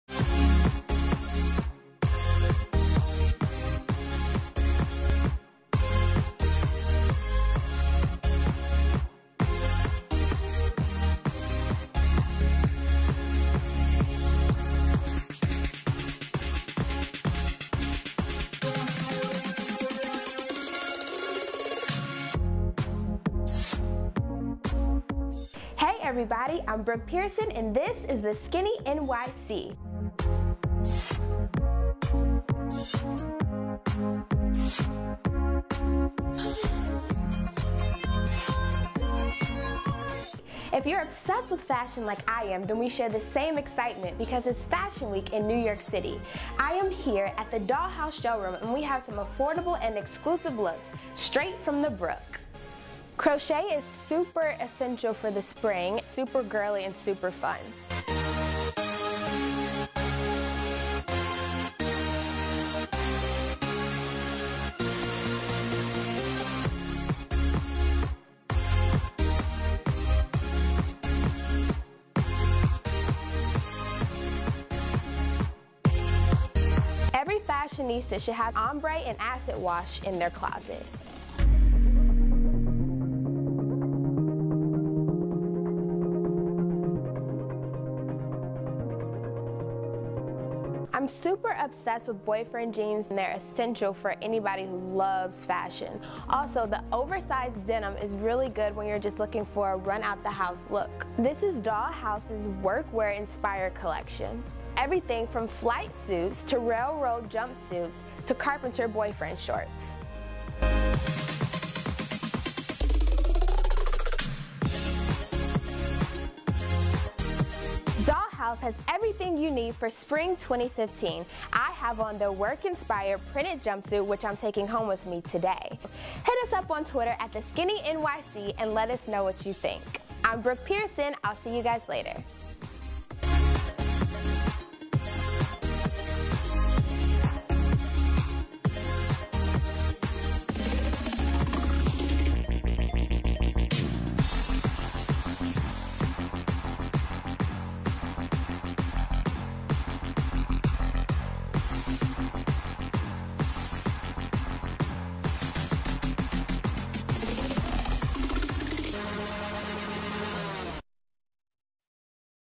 Spring in the Fall with Dollhouse Clothing
Brooke takes you on a advance look at Spring 2015 Fashion With Dollhouse